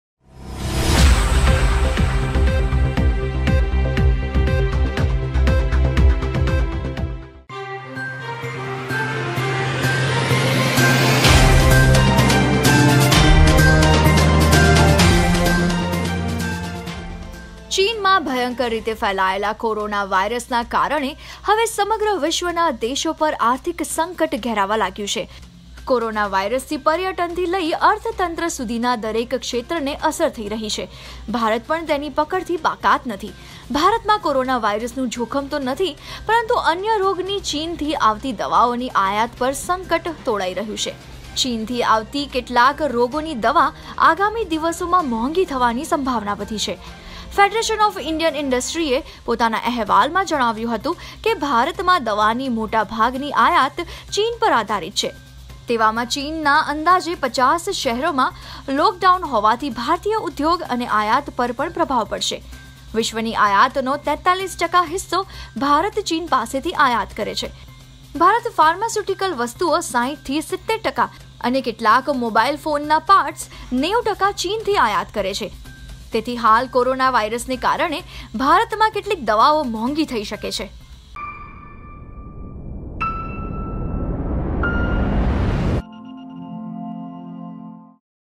કોરોનાના કારણે ચીનથી આવતી આ રોગની દવાઓ થશે મોંઘી ।। PTN News ।। Gujarati News
PTN NEWS

કોરોનાના કારણે ચીનથી આવતી આ રોગની દવાઓ થશે મોંઘી

Ptndigitalmedia.com